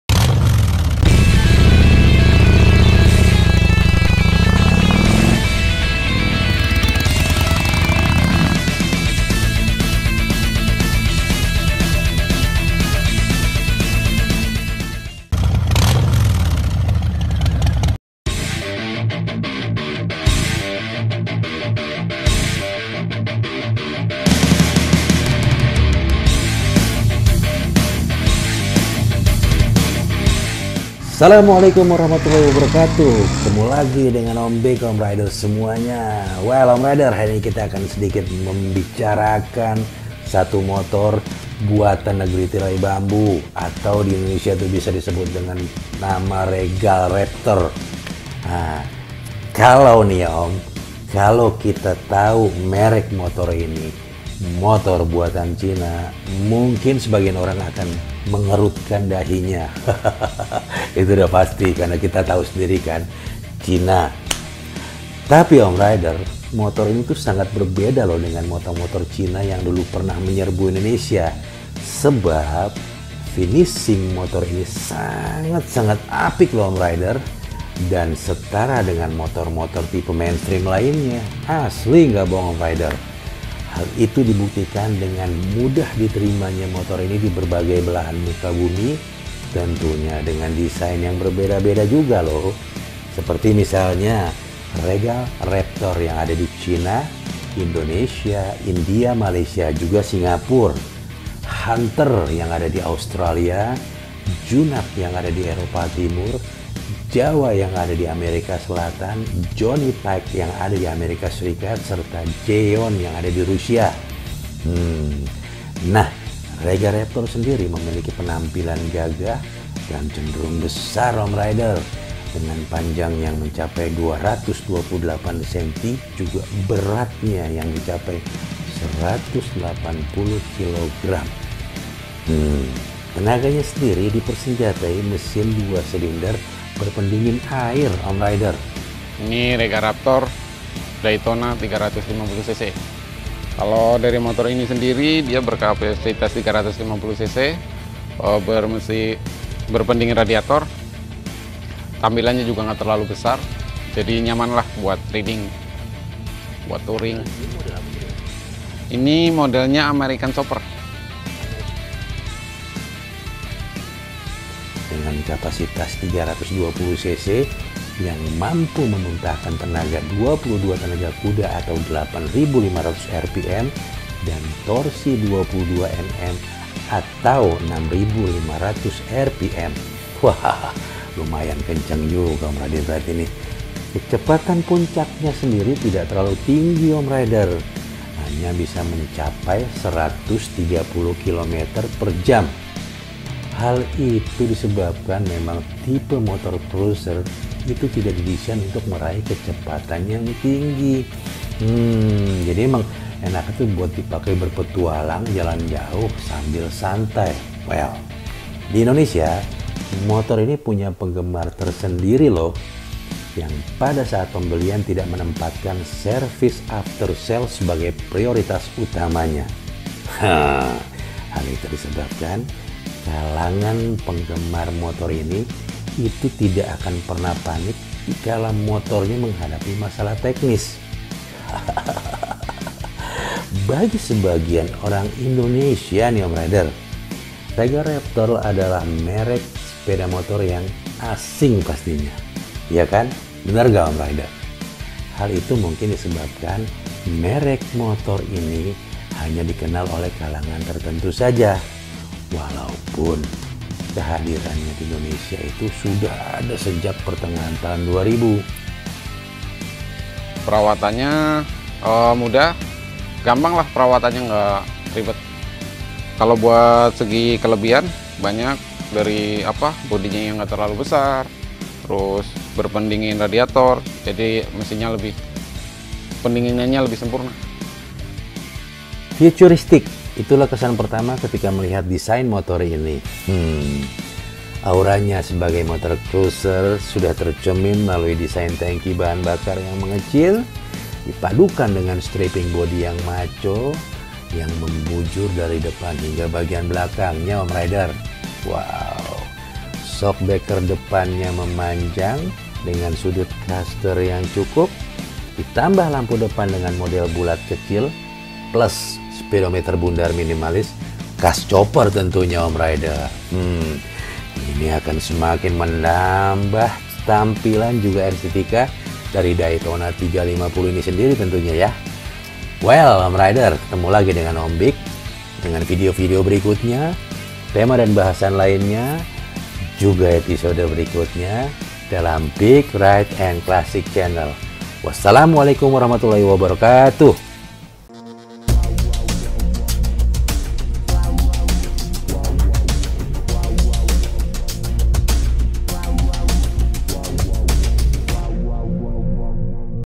Intip motor harley cina Regal Raptor Daytona 350
Futuristik, itulah kesan pertama ketika melihat desain motor harley cina ini. Auranya sebagai motor cruiser tercermin melalui desain tangki bahan bakar yang mengecil, dipadukan dengan stripping bodi macho yang membujur dari depan hingga belakang. Shock breaker depan memanjang dengan sudut caster yang cukup, ditambah lampu depan model bulat kecil plus speedometer bundar minimalis khas chopper semakin menambah estetika motor harley cina dengan nama regal raptor Daytona 350 tersebut